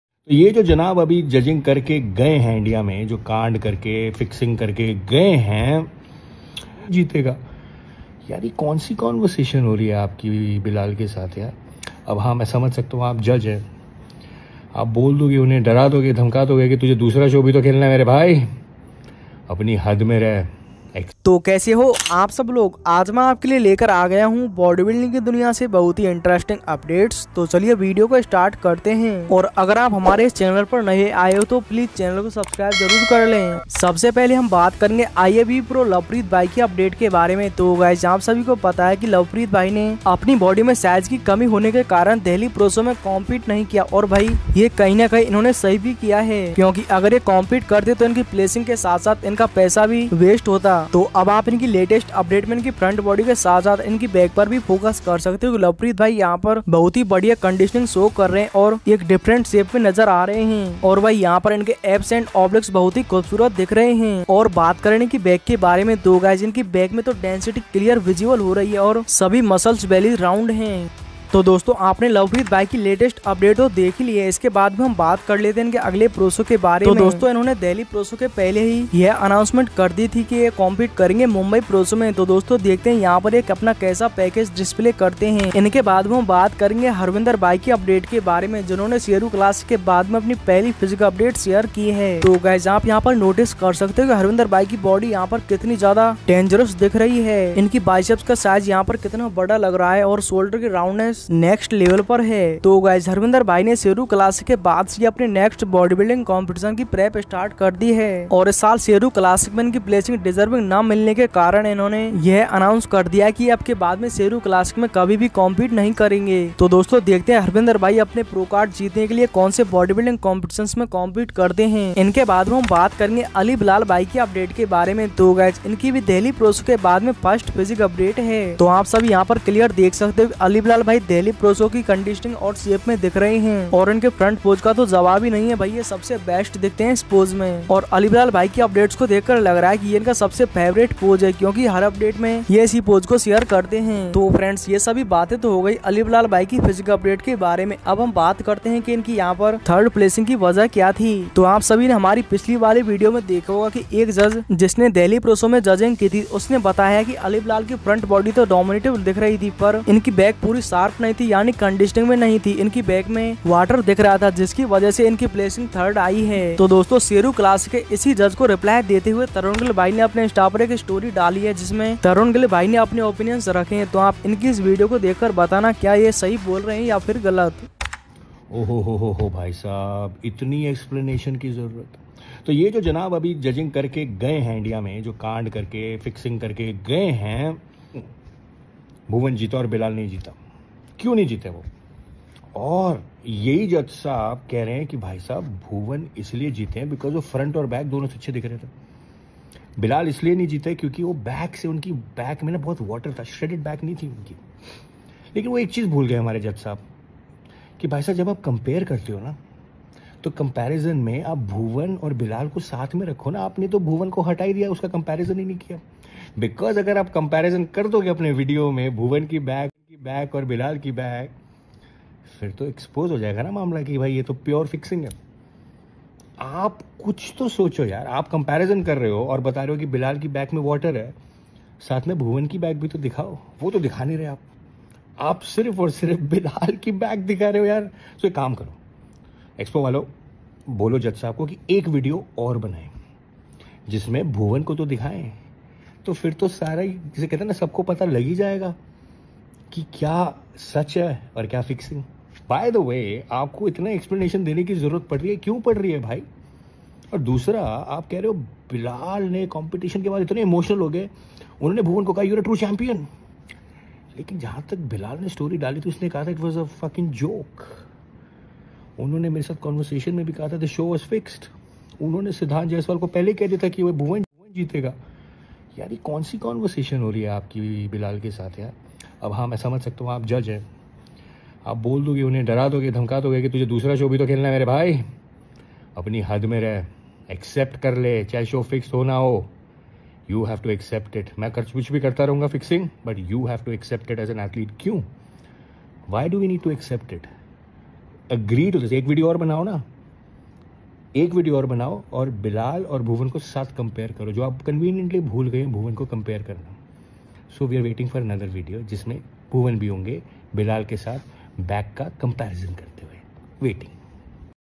Tarun Gill angry reply to the head judge of Sheru Classic 2O23. Ali Bilal Update After Sheru Classic
Tarun gill angry reply to ihff sheru classic, ali bilal letest physique update, ali bilal vs bhuwan chauhan in Delhi pro show 2O23, bodybuilding letest updates and news.